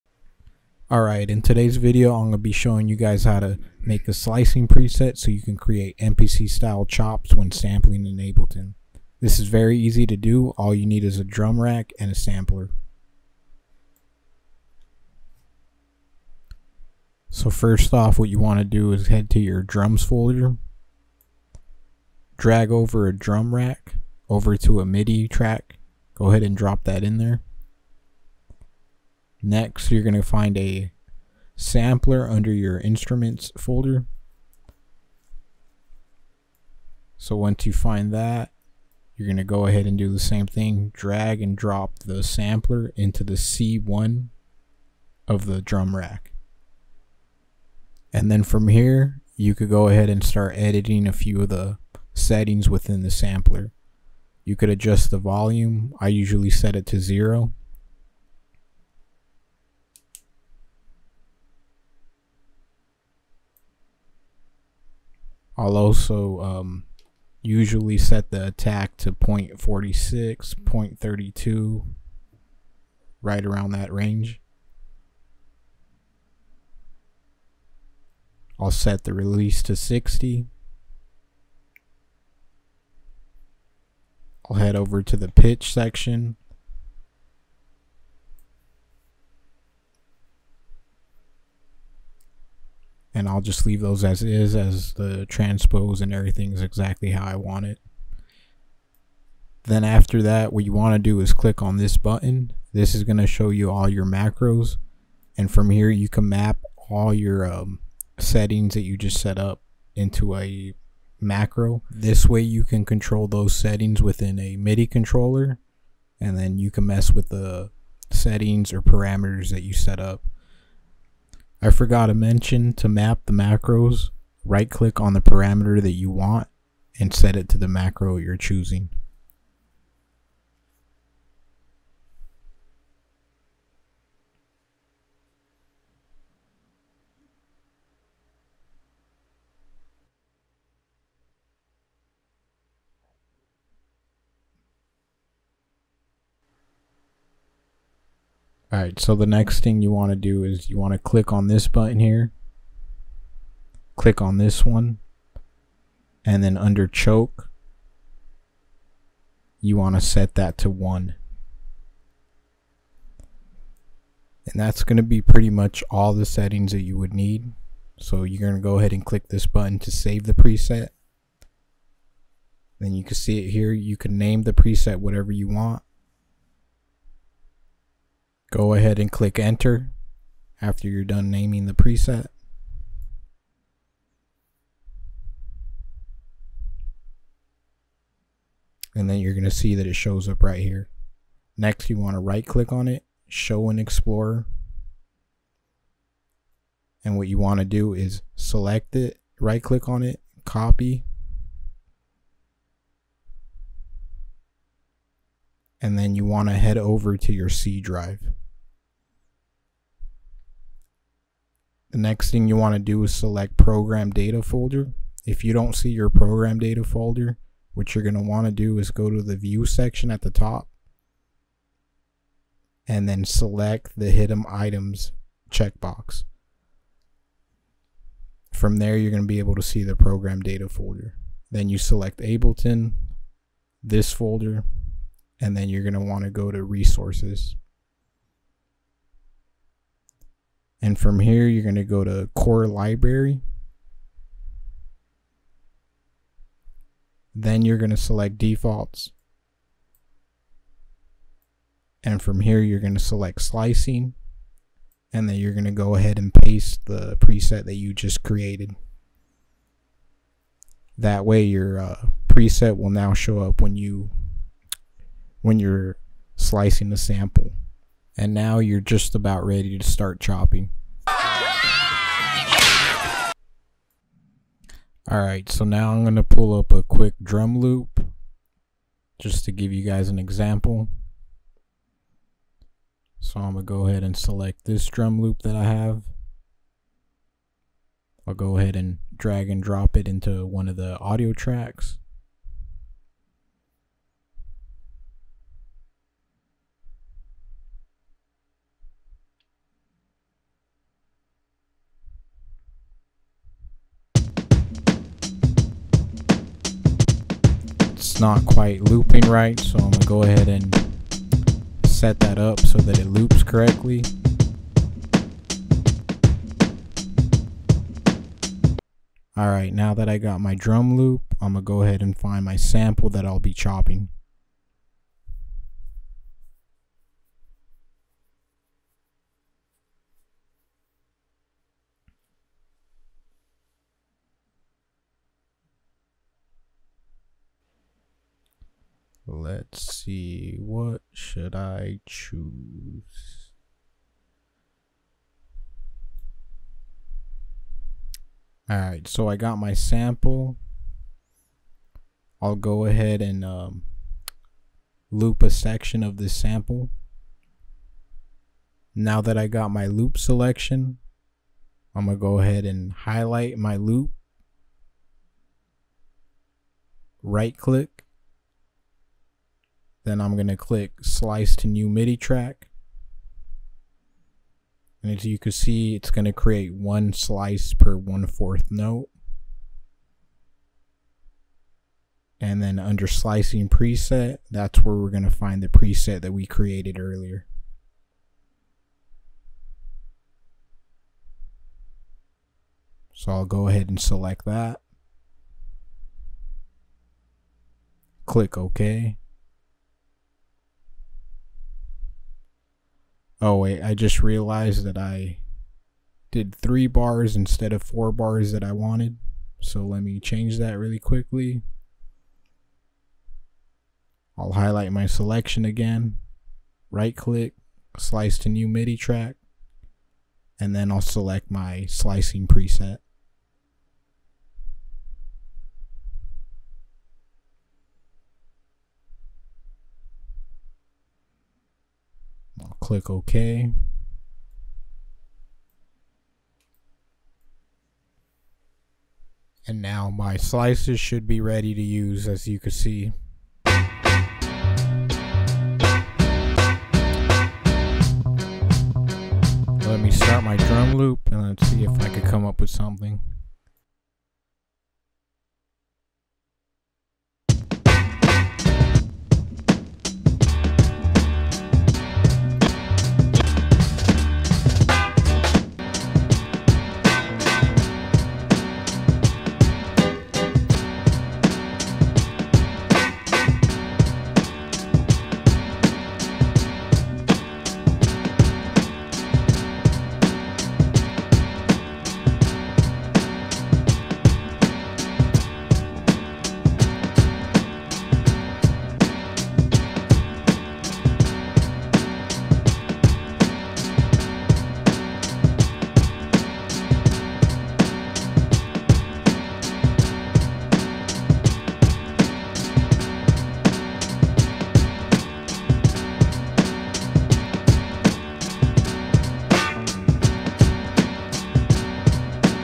How to Chop Samples Like a Pro in Ableton Live with Slicing Presets (Easy)
How I chop samples using a custom slicing preset that creates mpc style chops. I'll show you how you can create your own custom slicing presets,
Watch up until 4:20 you can skip the rest if you want.